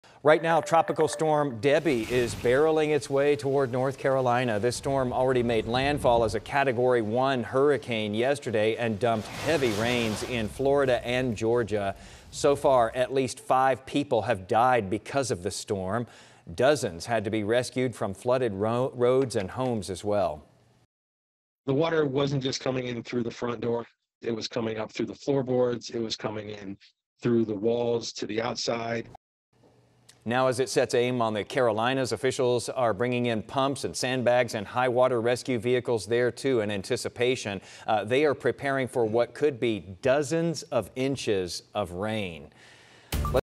Tropical Storm Debby: latest updates
Tropical storm Debby is heading toward North Carolina after making landfall in Florida as category 1 storm.